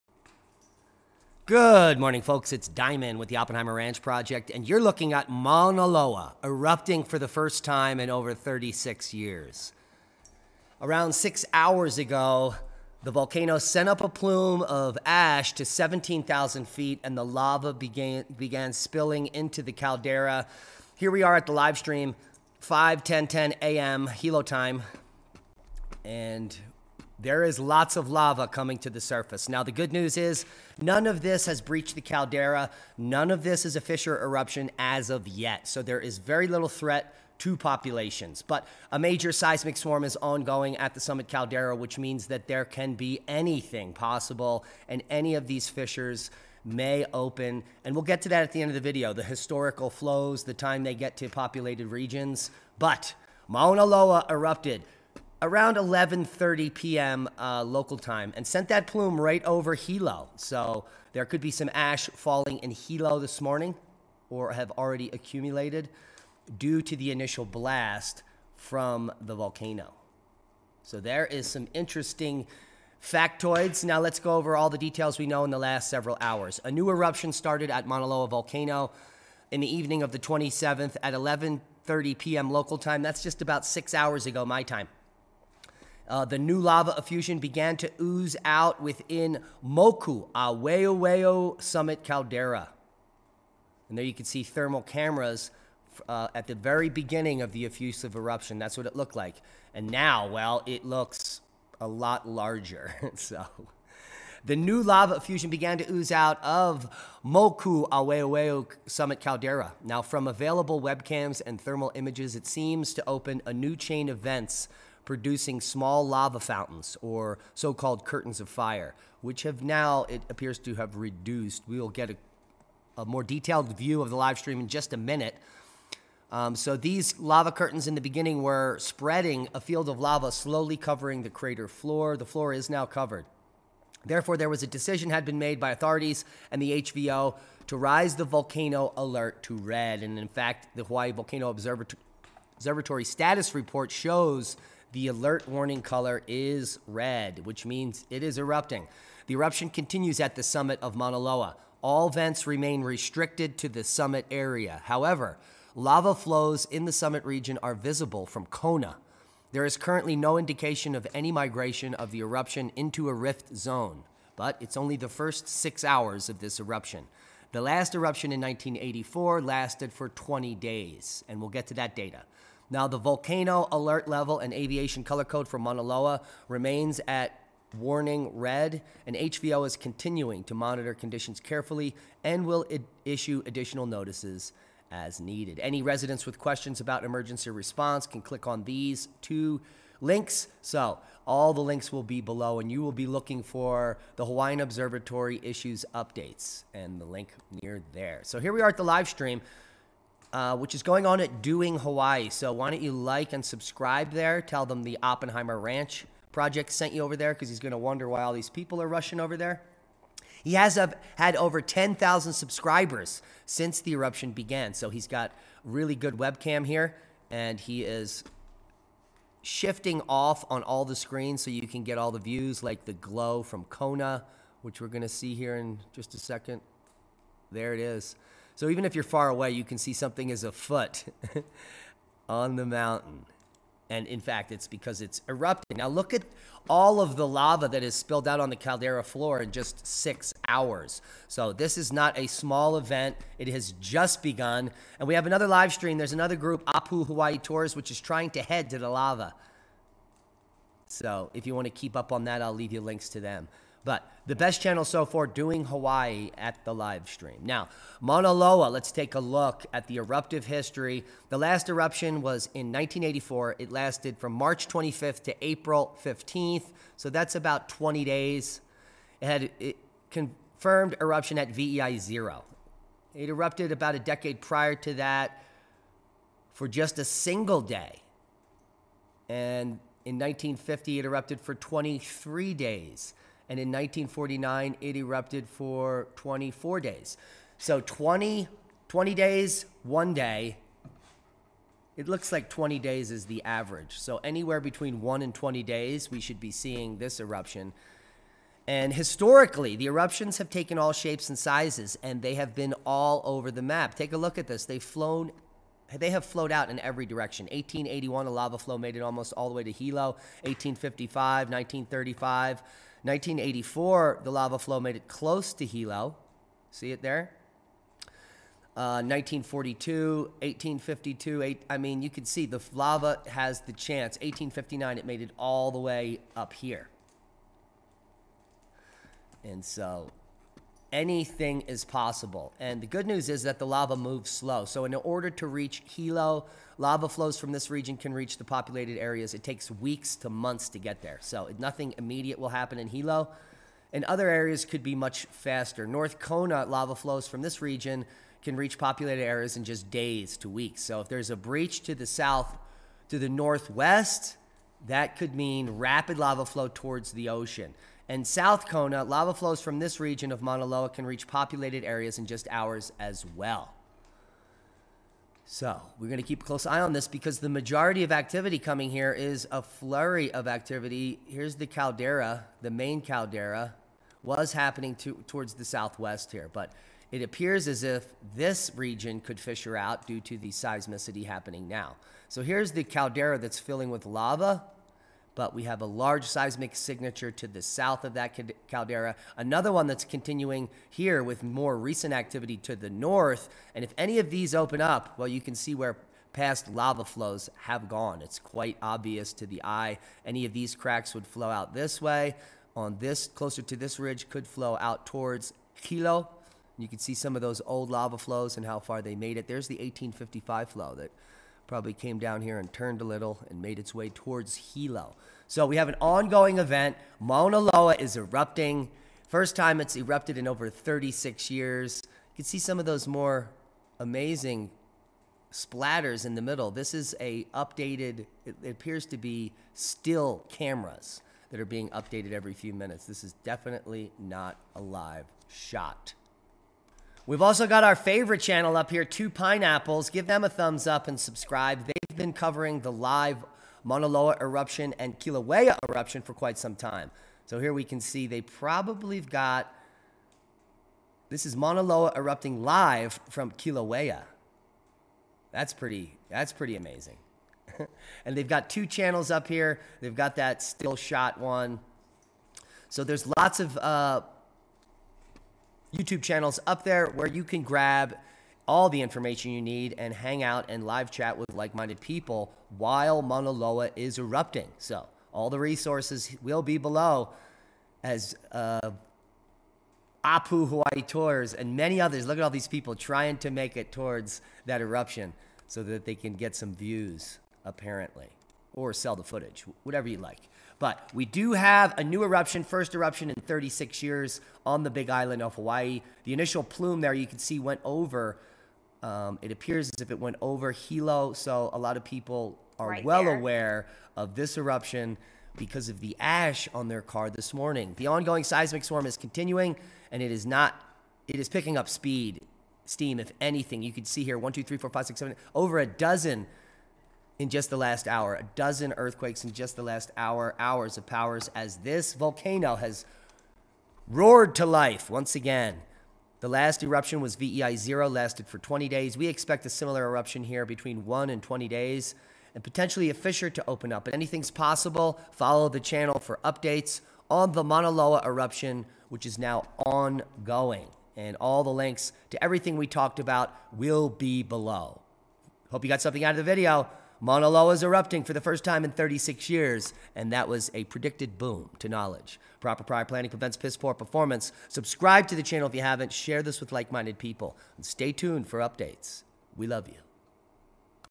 Mauna Loa Volcano Erupts To 17,000ft In Hawaii After 38 Year Hiatus - Full Coverage And Livestreams
Maunaloa Volcano (Big Island, Hawai'i): new eruption in summit caldera, alert status raised to Red

Bitcoin: 17NV3vtcQhRnsRnjckCcYkKVEUcnirKQ62

Bitcoin: 17NV3vtcQhRnsRnjckCcYkKVEUcnirKQ62